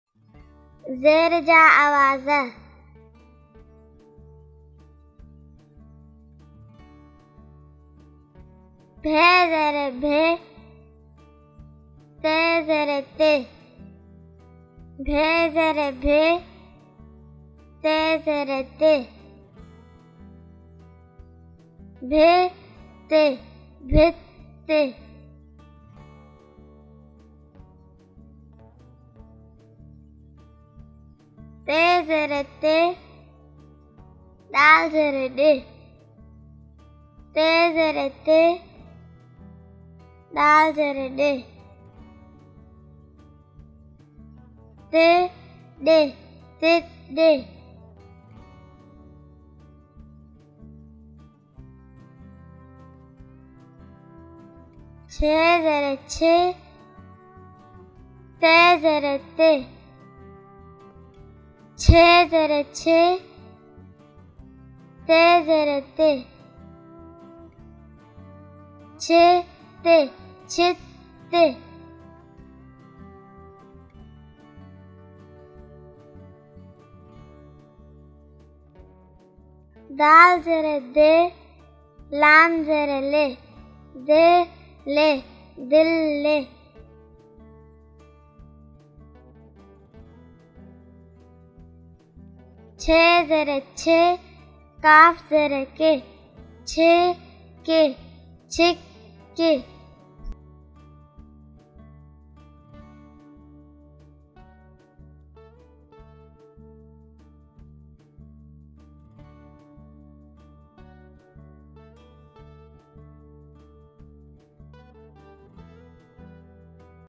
Zerr Ja Awaz ڇت ڀت Learn Sindhi
In this video you will learn sounds of Zerr of the Sindhi words with two letters.